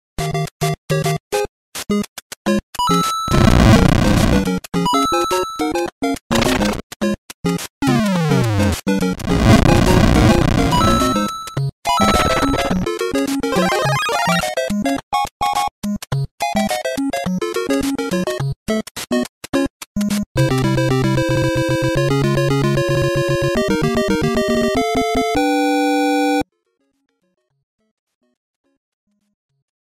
My little gameboy
Dad made a game out of the ordinary video of his son.